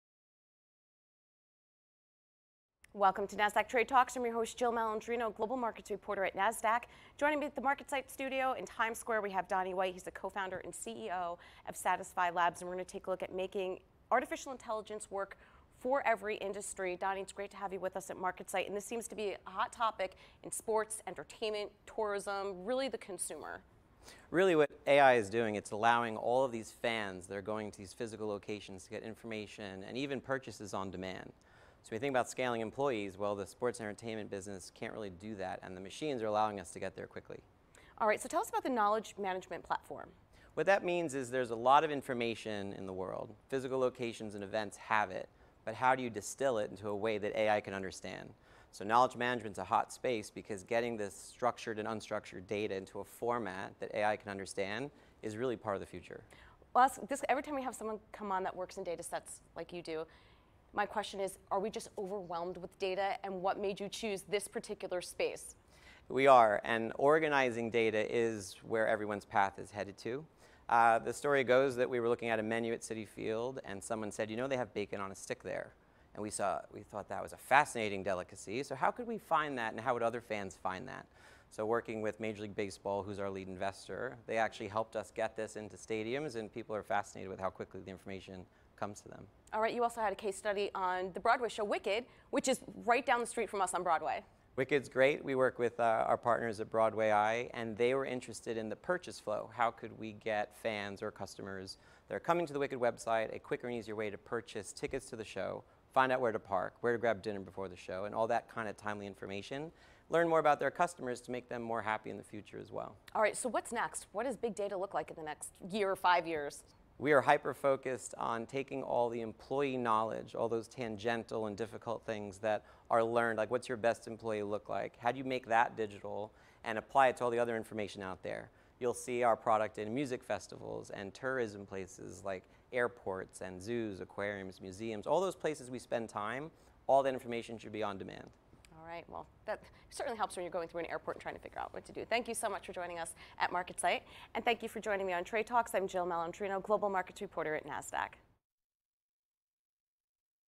Making AI Work for Every Industry
Don White is the CEO and Co-Founder of Satisfi labs and he sits down to discuss how AI affects the sports and entertainment industries.

Follow Trade Talks
Follow our host: Jill Malandrino
Follow NASDAQ
Follow Our Guest: Satisfi Labs